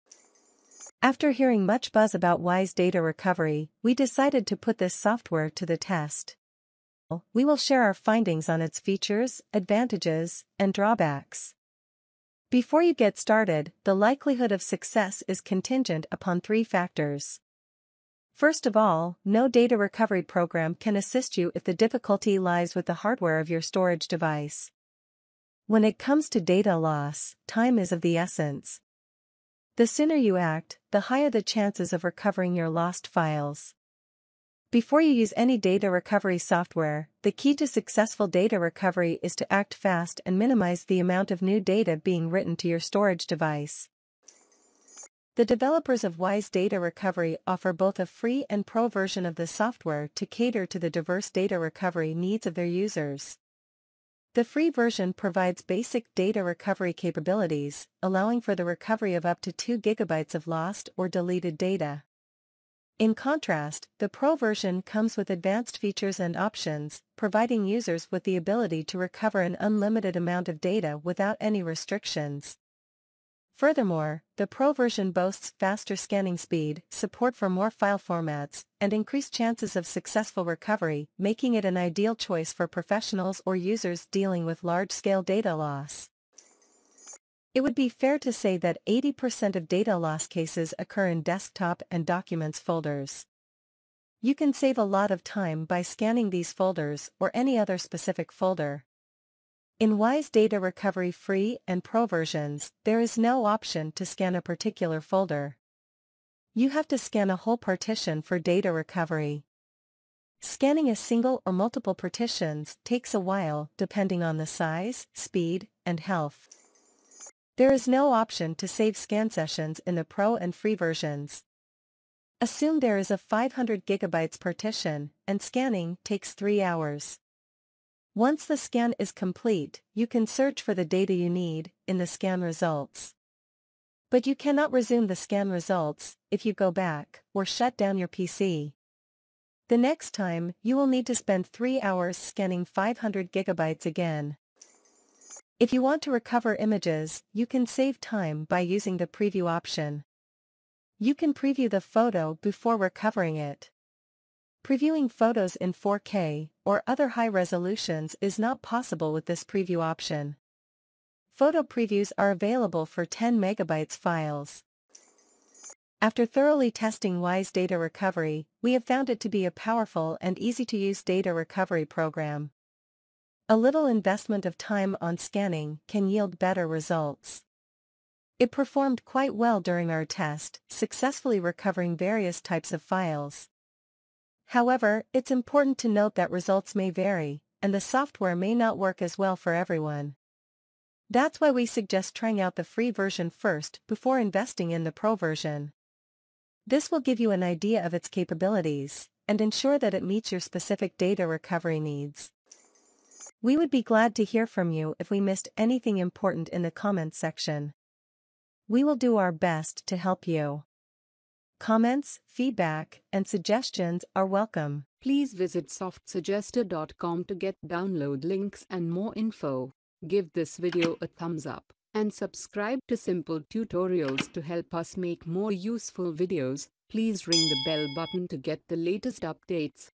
Wise Data Recovery Pro/Free Review: Uncovering Its Features, Advantages, and Drawbacks
This video reviews Wise Data Recovery software and examines its features, advantages, and drawbacks. The likelihood of successful data recovery is dependent on the hardware of the storage device and the speed at which action is taken. Wise Data Recovery offers both a free and Pro version, with the free version allowing for the recovery of up to 2GB of data and the Pro version offering unlimited data recovery and advanced features. However, the software does not have the option to scan a specific folder and does not allow for saving scan sessions. The preview option is available for recovering images but is limited to 10MB files and does not support high resolution previews.